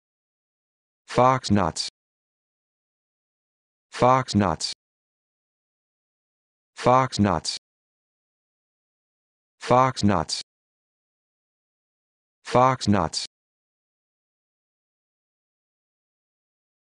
How to Pronounce Foxnuts
This video shows you how to pronounce Foxnuts